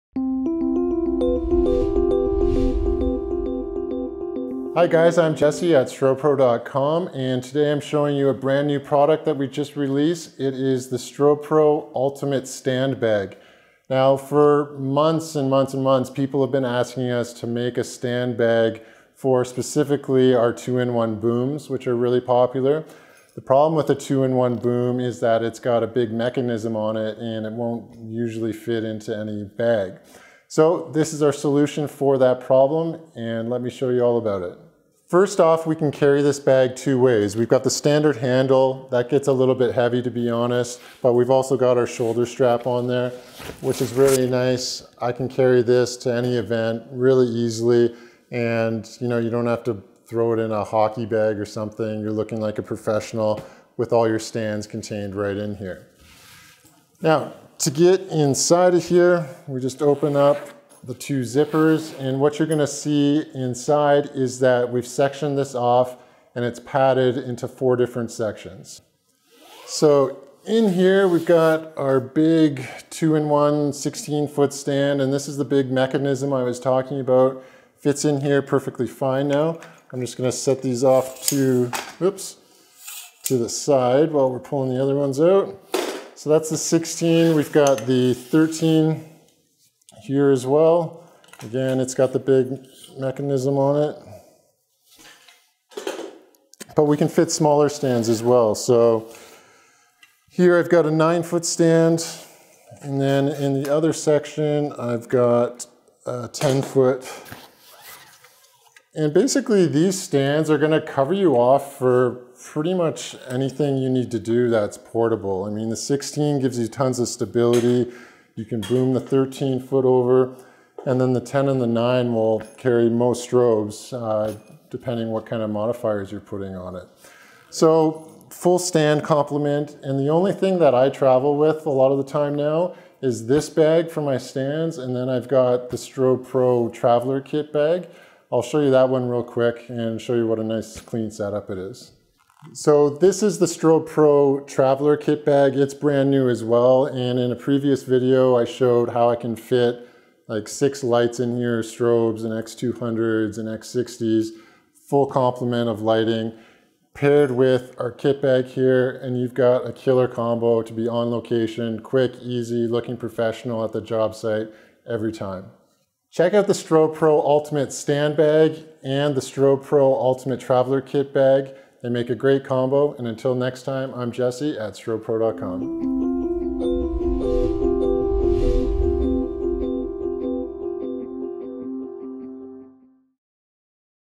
Strobepro Ultimate Stand Bag
EASY STAND TRANSPORT
Protect your stands and carry them with ease in the Strobepro Ultimate Stand Bag.

FITS FOUR STANDS
The Ultimate Stand Bag will carry four stands up to 48" tall.  The inside has dividers permanently attached to perfectly separate your stands into sections.

PADDED PROTECTION
The entire bag is padded to protect your stands from scratching and banging around.

LOOK THE PART
You're a professional so show up to your next shoot looking like it.  Clean, organized and ready to go.  No more looking everywhere for your stands.

SIZE
48" long x 10.5" diameter